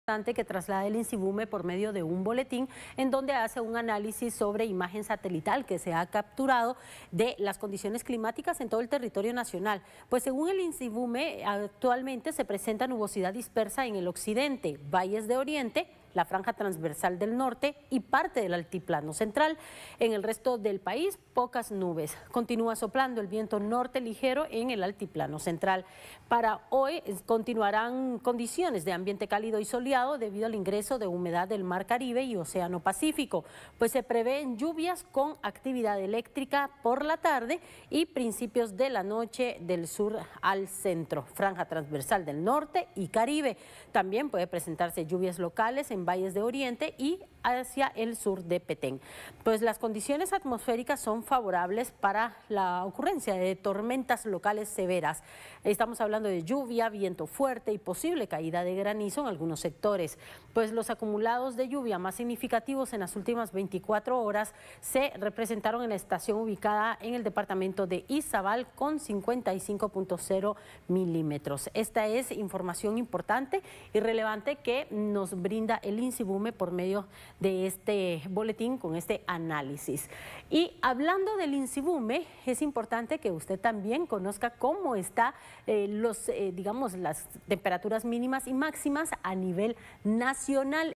Estas son las condiciones climáticas para las próximas horas
Se esperan lluvias con actividad eléctrica

En su más reciente informe el INSIVUMEH dio detalles sobre las condiciones climáticas para las próximas horas en el país. Visita nuestro sitio